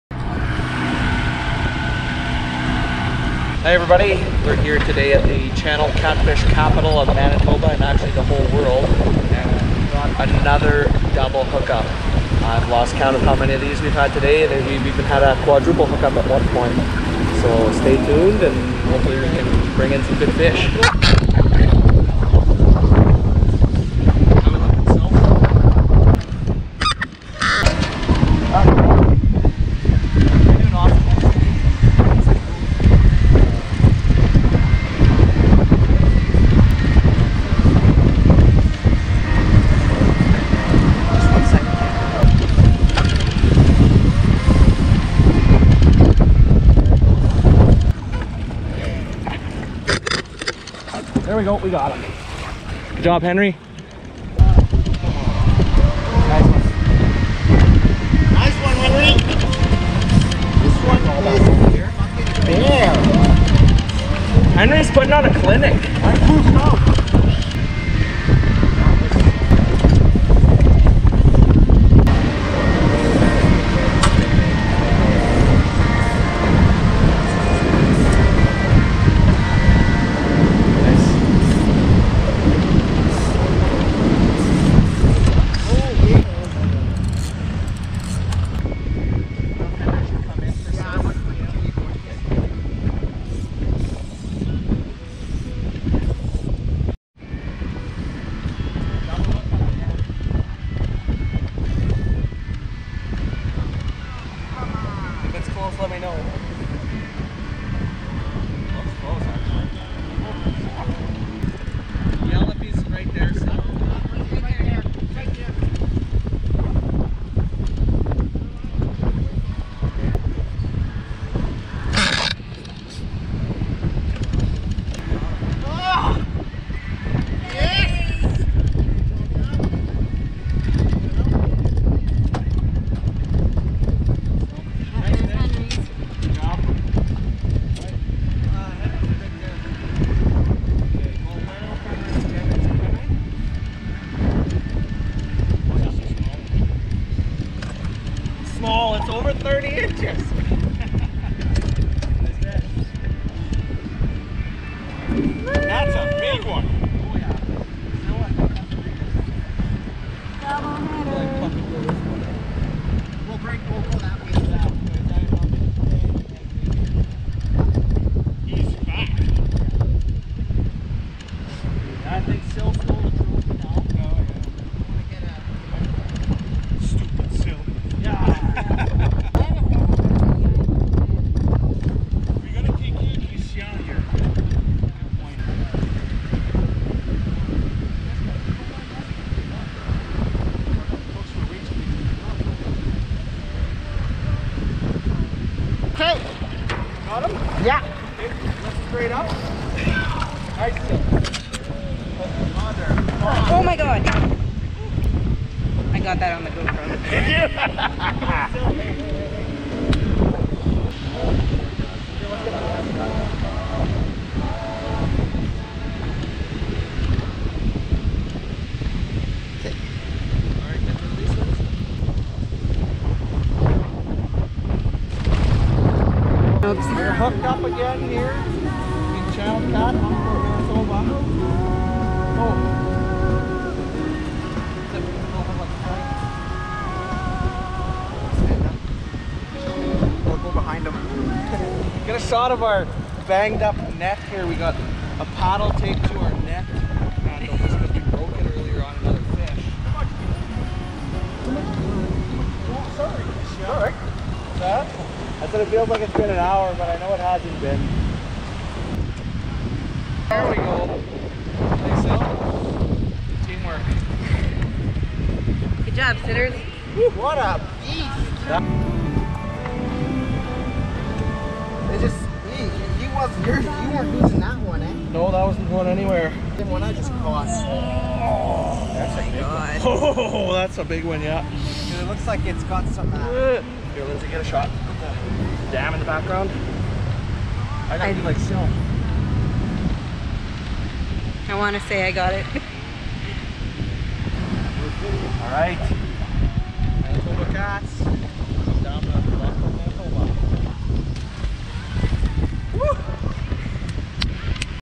Catfishing at Lockport Henri’s “make a wish”
Catfishing at lockport, manitoba on the red river below the saint andrews dam on my buddy Syl's pontoon boat.

My friend Henri asked us to take him out catfishing for trophy channel cats